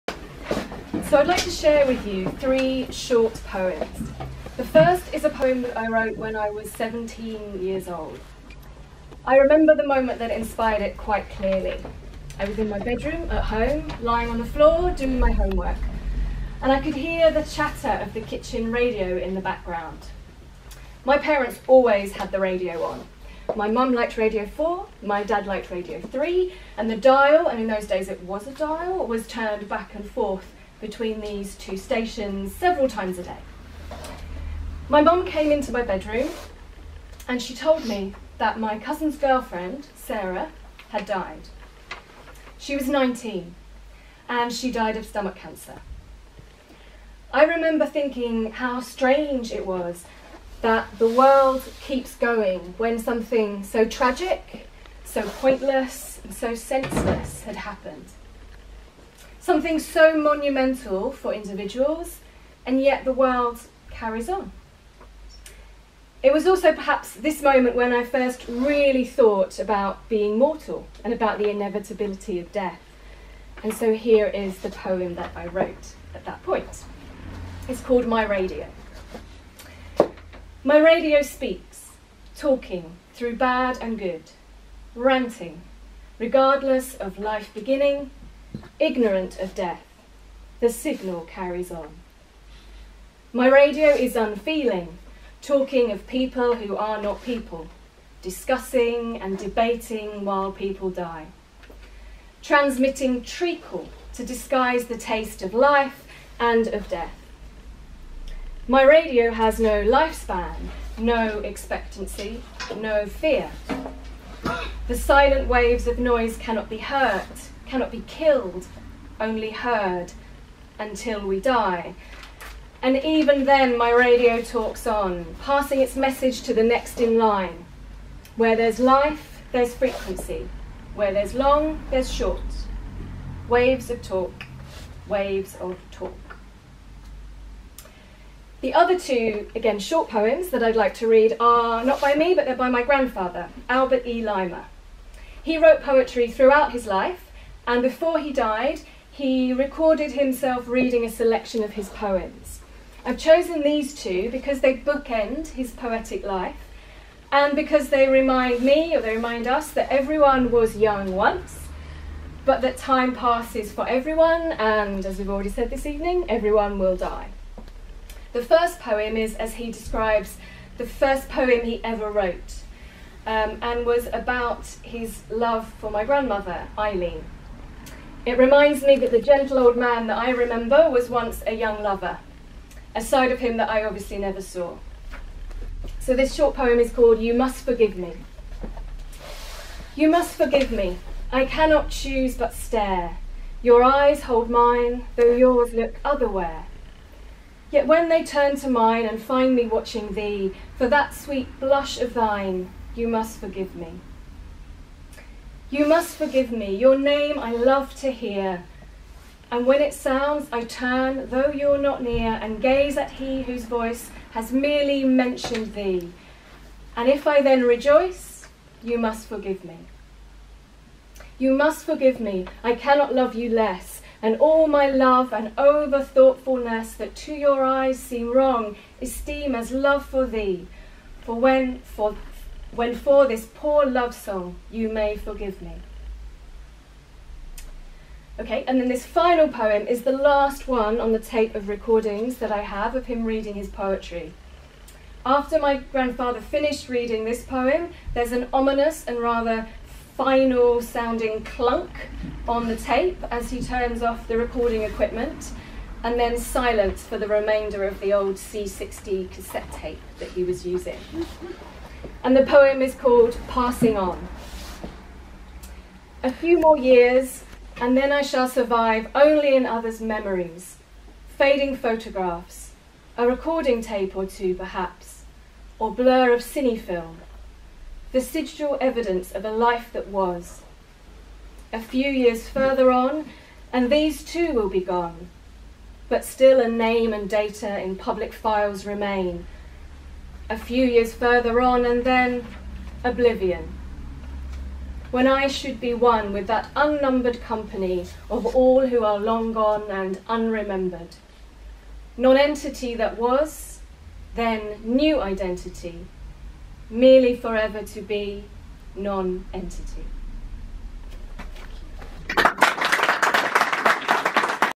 Kate Scott - Memento Mori : Writers Centre Kingston
Memento Mori : on Mortality
November Tuesday 19th 2019 at Museum of Futures A themed literary happening on remembering our mortality, and the benefits of death remembrance.

This event resented speakers from a multitude of backgrounds and disciplines, from the staff of Kingston University to local writers and visiting speakers. Their responses to the theme will be positively various and can include new readings of fiction and poetry alongside experimental or theatrical performance, speeches, talks, recitals and everything in between, excluding academic lectures.